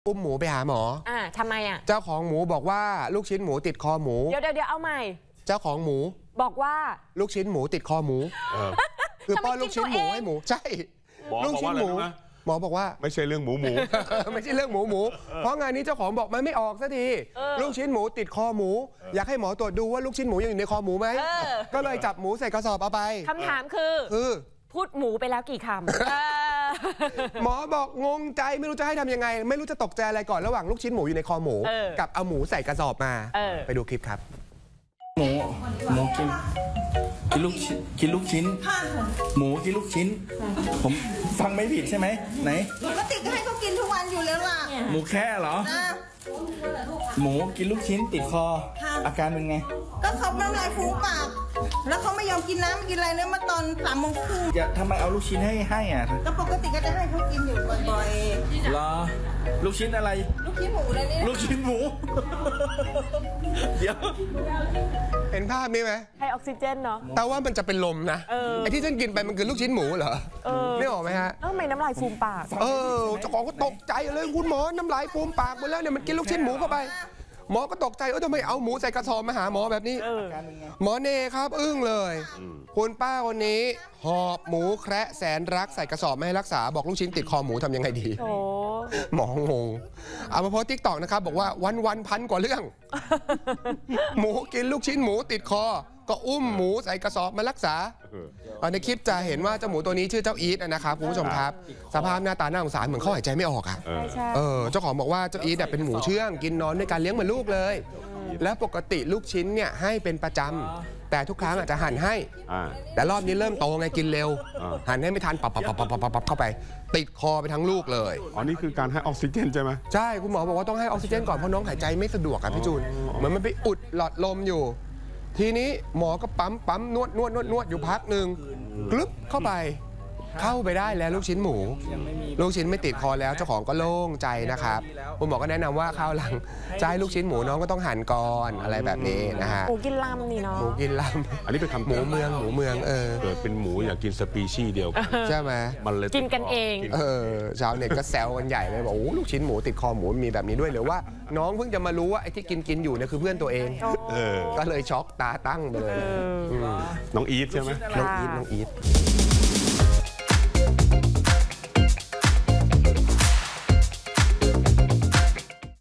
อุ้มหมูมาหาหมอ! หมูกินลูกชิ้นหมูติดคอ | ข่าวช่อง8
เจ้าของรีบเอาหมูใส่กระสอบ มาให้สัตวแพทย์รักษา หลังให้หมูกินลูกชิ้นหมู ทำให้ลูกชิ้นหมูไปติดคอหมู คุณหมอเจอแบบนี้เข้าไป ไม่รู้จะอึ้งอะไรก่อนดี